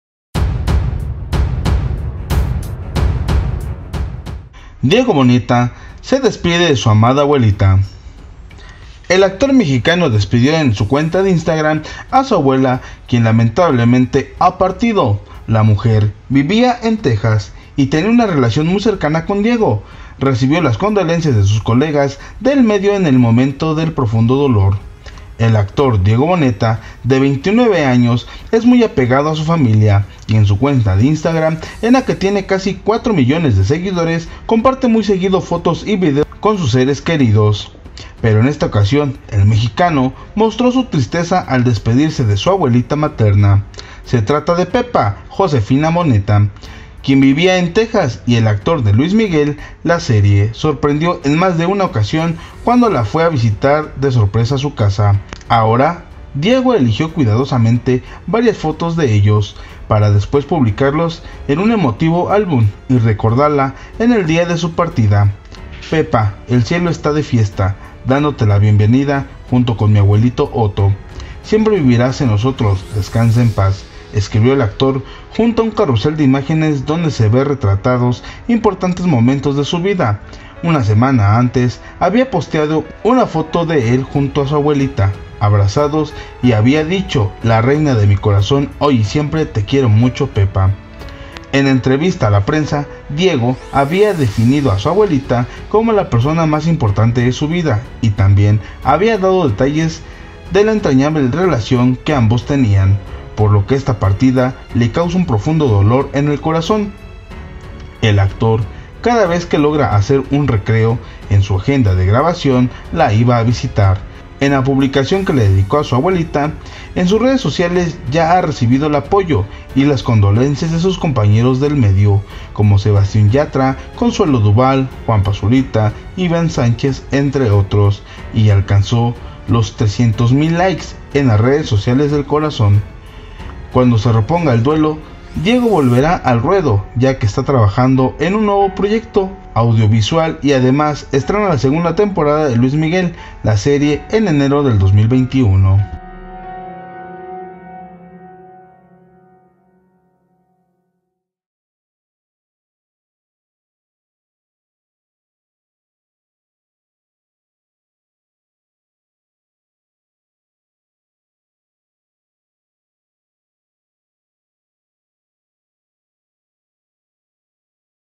El Dolor Inunda a Diego Boneta tras Perder a su Abuela.
pierde a su ser querido Diego boneta